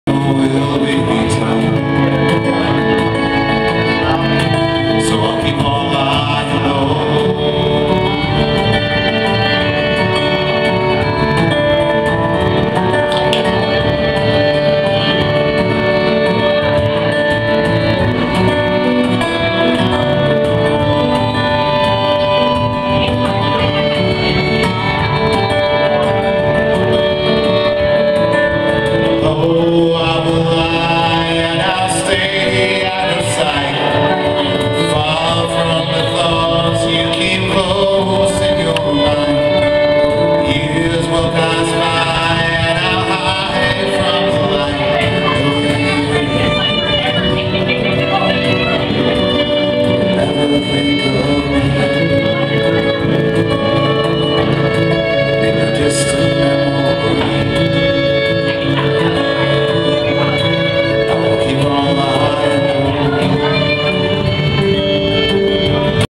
Tobias the Owl at Big Bldg Bash 2015
Tobias the Owl playing Studio 18 late in the afternoon on June 6, 2015